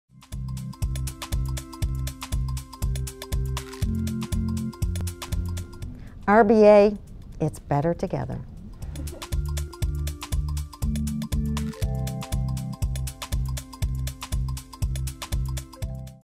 You Will Not Believe Why Judy Uses RBA...
Judy Chalmer, the Executive Director of Vermont Special Arts uses and believes in the power of Results Based Accountability. Here is her quick tip on how to be successful when implementing RBA in your organization.